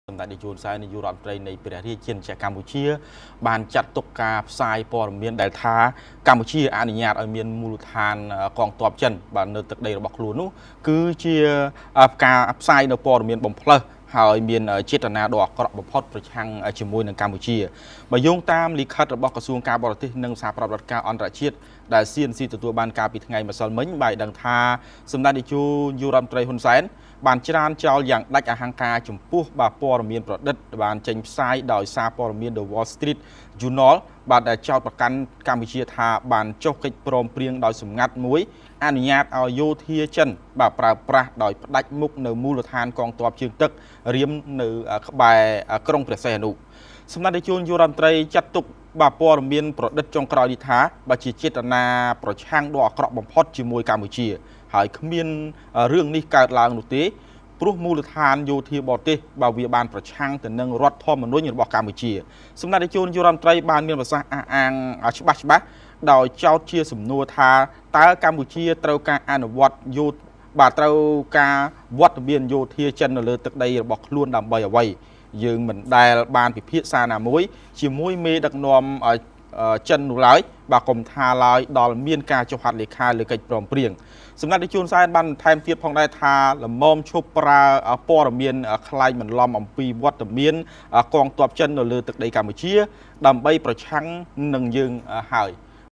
សម្តេចតេជោចាត់ទុកការផ្សាយក្លែងបន្លំ គឺមានចេតនាដ៏អាក្រក់បំផុត
សម្តេចតេជោ ចាត់ទុកការផ្សាយព័ត៌មានថា កម្ពុជាអនុញ្ញាតឲ្យមានមូលដ្ឋានយោធាចិននៅទឹកដីខ្លួន គឺមានចេតនាដ៏អាក្រក់បំផុតប្រឆាំងជាមួយកម្ពុជា